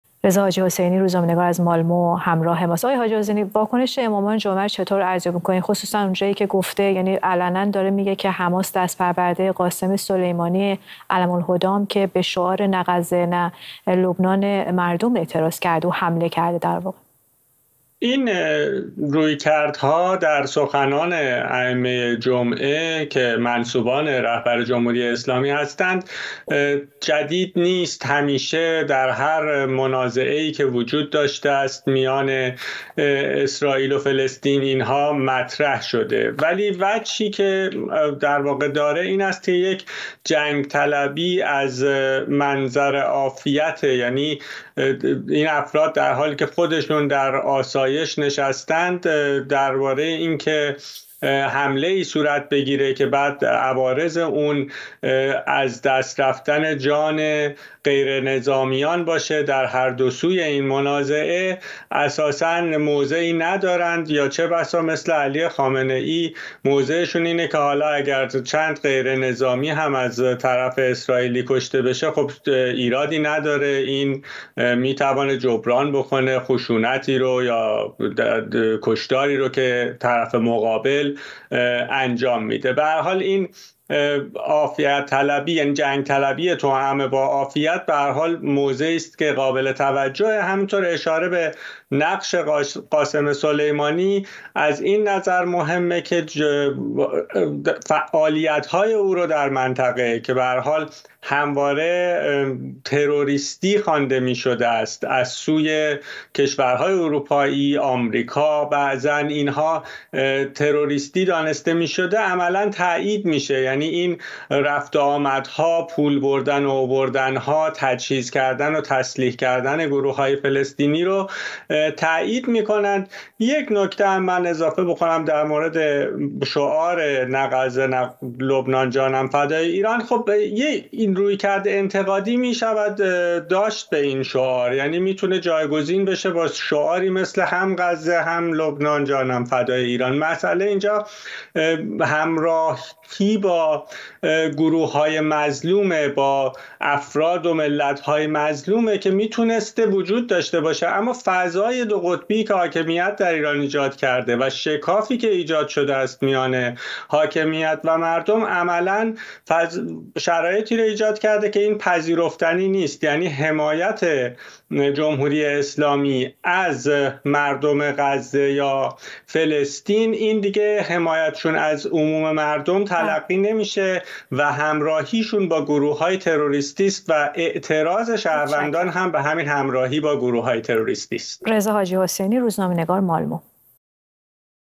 واکنش امامان جمعه در ایران به جنگ حماس و اسرائیل
گفت‌وگو با رضا حاجی‌حسینی، روزنامه‌نگار